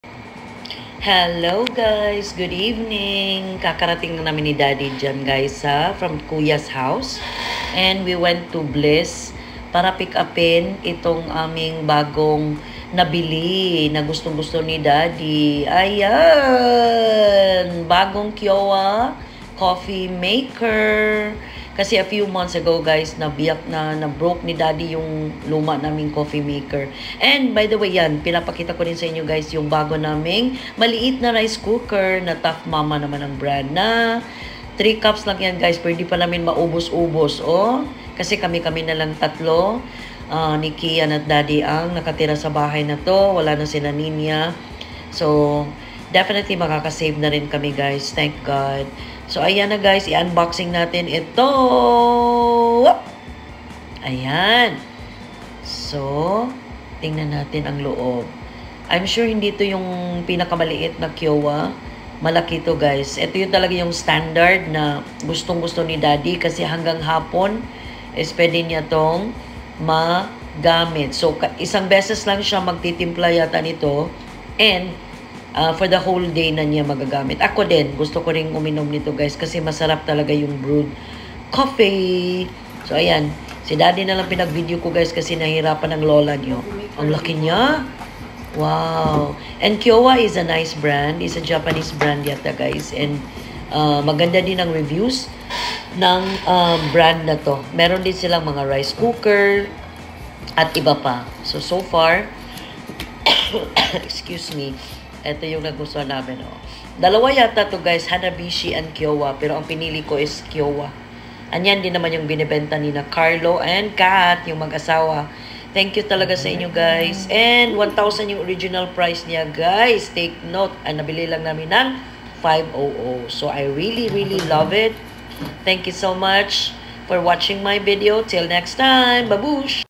UNBOXING NG NEW COFFEE MAKER namin+HAPPY SI HUBBY
Wazzzappp guys!! Today is nag unboxing kami nitong bagong KYOWA Coffee Maker na nabili namin kina Carlo and Cath na mga kabarkada ng anak ko si Kuya Ken! Ang saya ni Daddey dahil gustong gusto talaga nya ng coffee maker.